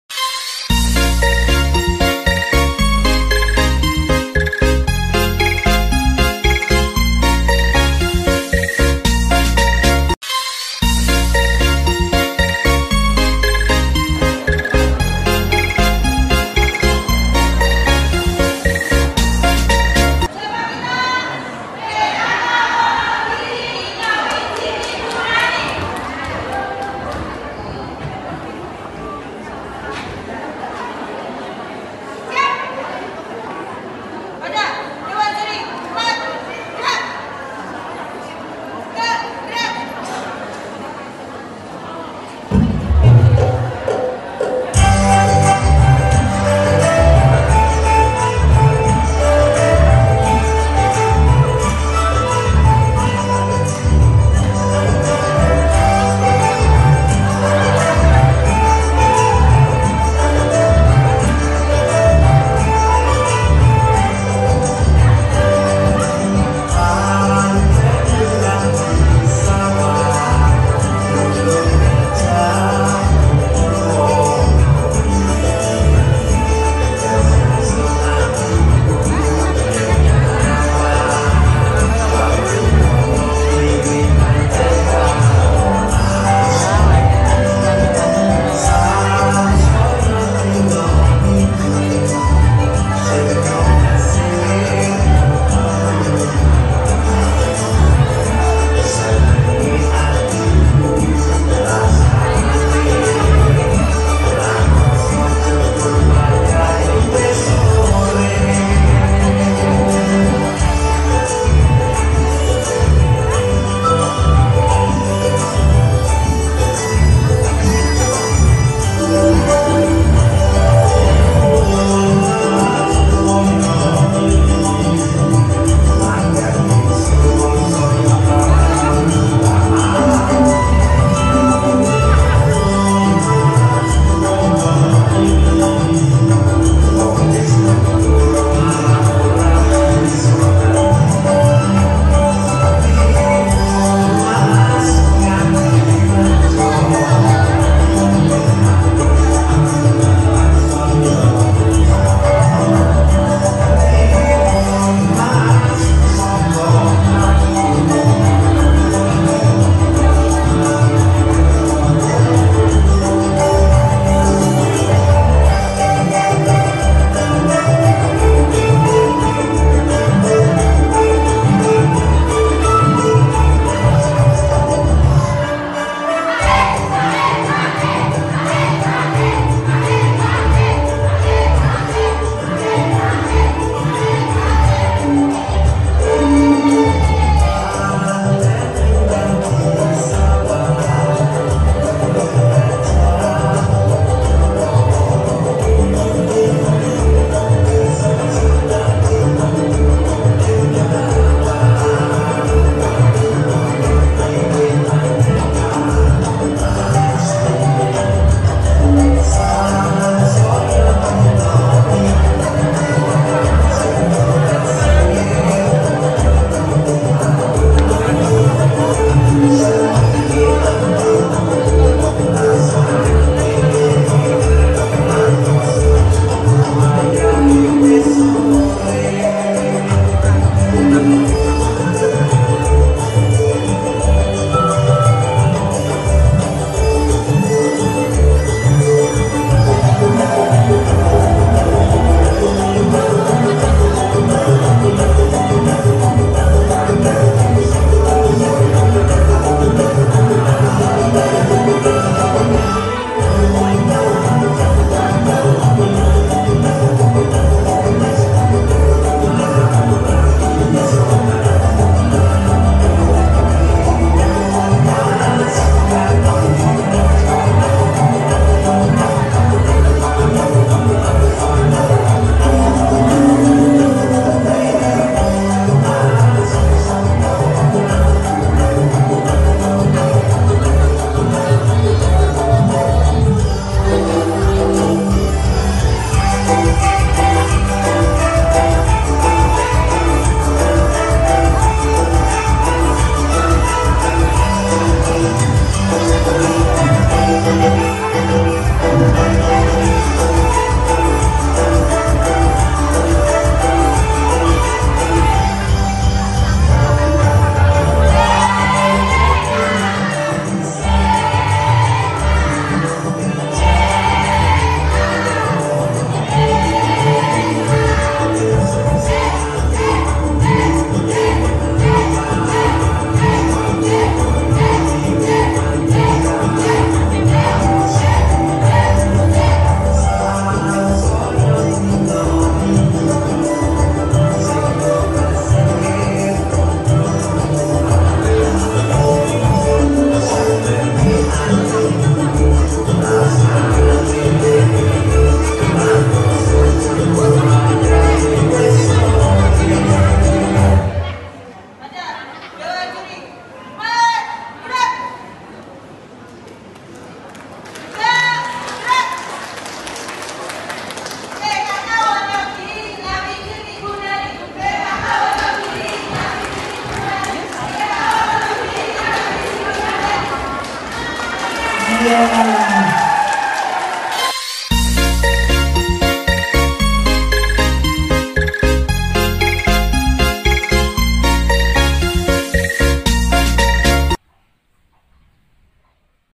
LOMBA SENAM KREASI 6 LANGKAH CUCI TANGAN..‼️TIM KAB WONOGIRI#lomba
LOMBA SENAM KREASI 6 LANGKAH CUCI TANGAN..‼️TIM KAB WONOGIRI
Lomba tingkat propinsi Jawa Tengah
Semarang, Tgl 5 Maret 2024
Yang di wakili oleh PKK  kec. NGUNTORONADI kab. Wonogiri

Di ikuti oleh 33 tim peserta lomba.

lomba
senam
senam kreasi
senam cuci tangan 6 langkah
pkk
NGUNTORONADI
Wonogiri
Semarang